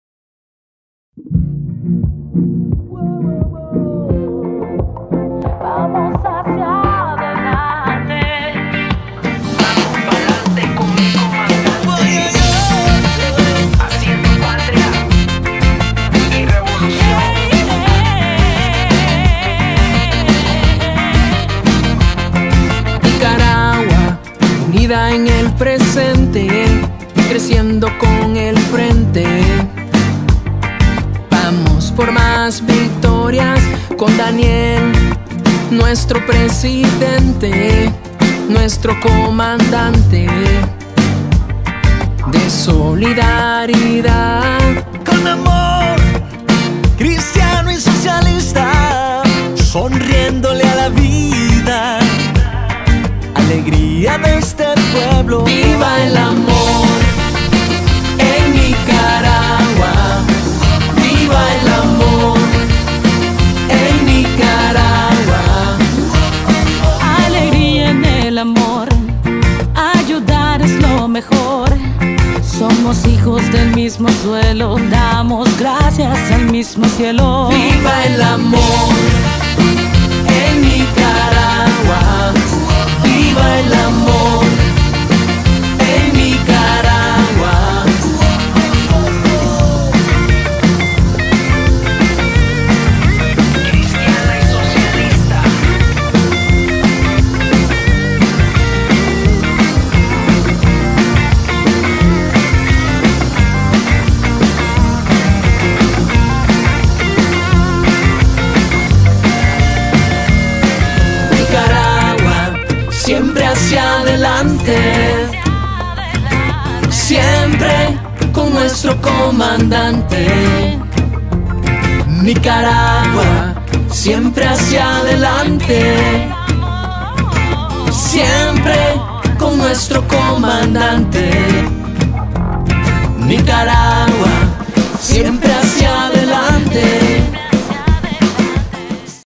Nicaragua 🇳🇮 Instalación de U.V.E Nora Astorga Distrito III Managua
Instalación De U.V.E. Distrito III Managua commemorando el 44 Aniversario del Paso a la Inmortalidad del Comandante y Jefe de la Revolución Sandinista Comandante Carlos Fonseca Amador Tayacan Vencedor de Muerte